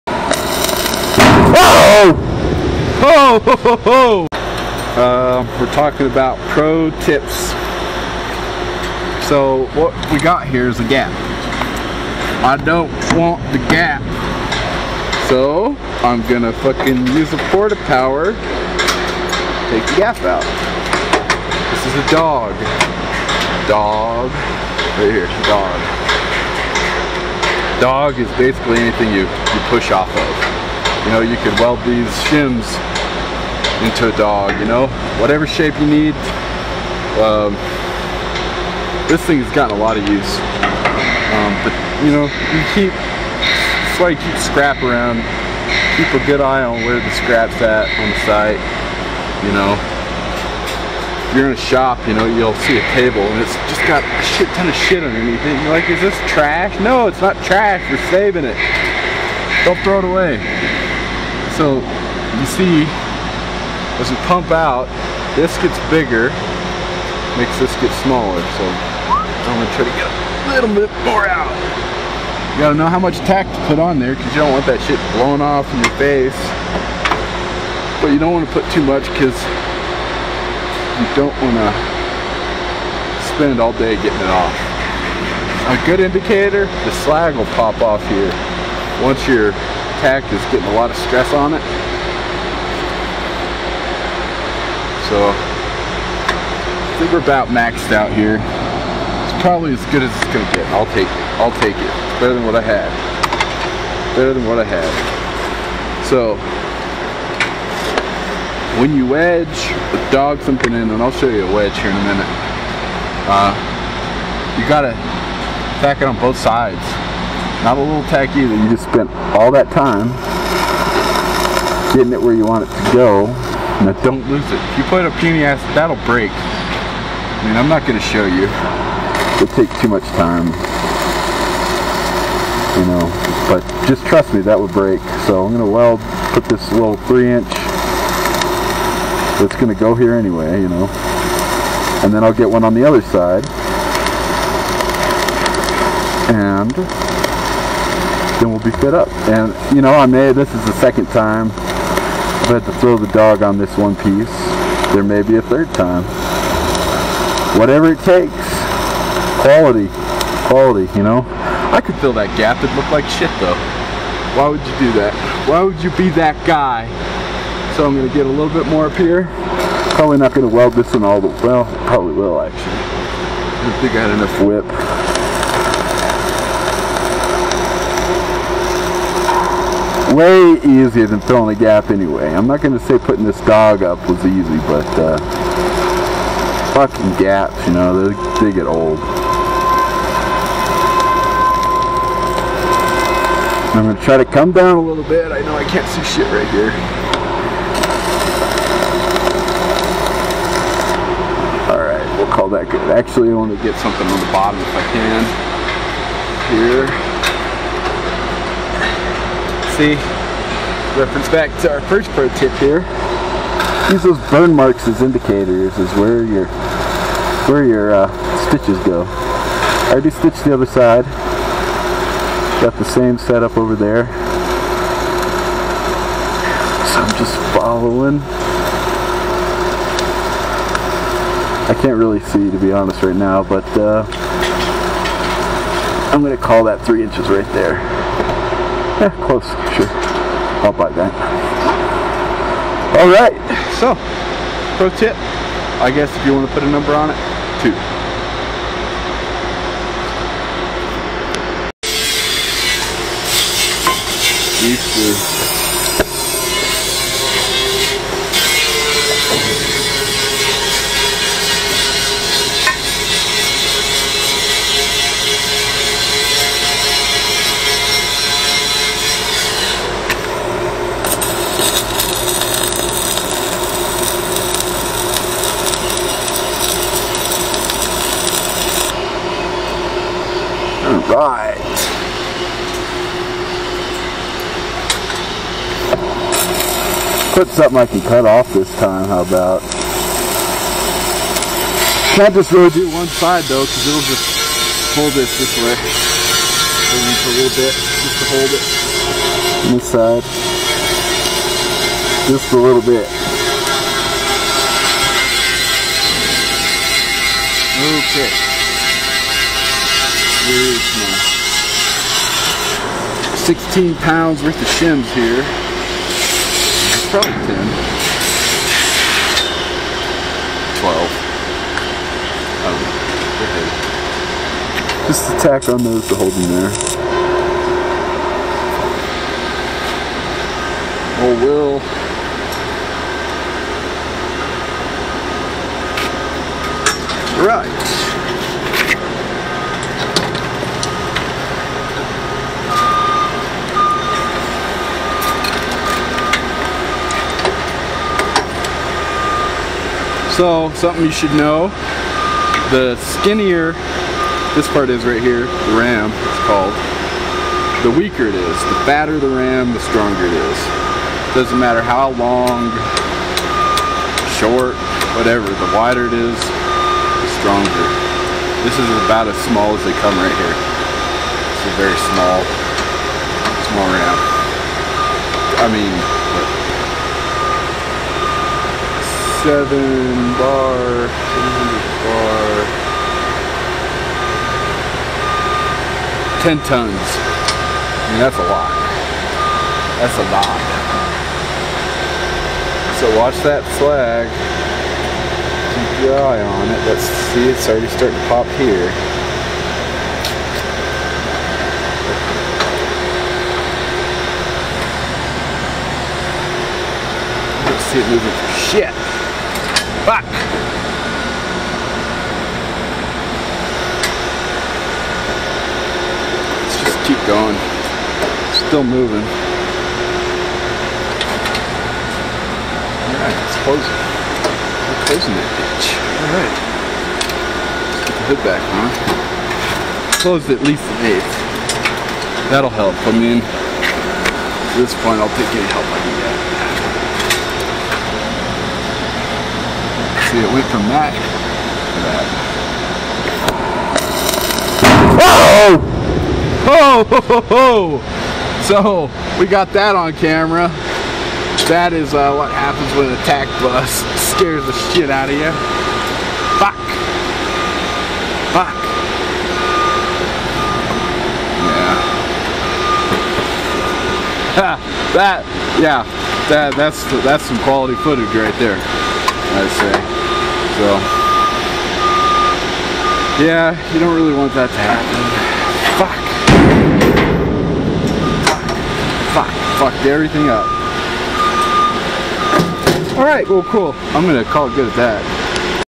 Welding Pro Tips- dogs and wedges, getting it to fit
more often than not, when fitting material up to weld, you are gonna have to use a little persuasion. in todays pro tip, i show you a few ways to attach, and monitor your dogs (point to push from, weather a porta power or wedge)